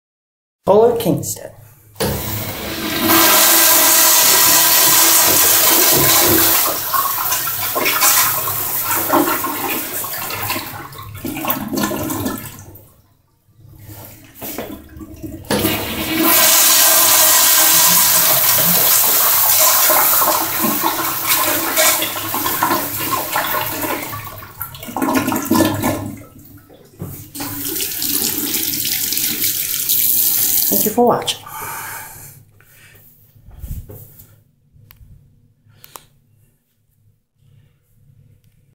Bathroom Tour: 1990s Kohler Kingston Waterguard at an Elementary School
Here is a reupload of my old bathroom video from November of 2018 or so not sure of the date

Anway this was filmed at my brother's former elementary school known as Mary Tisko . This is the staff bathroom while I was voting in the general elections at that time.

This toilet is the best so far out of all the fixtures that I found previously. It beats the 1.6 Kissytraps anyday, These do not clog.

Toilet: Early 90s Kohler Kingston Waterguard (10/10)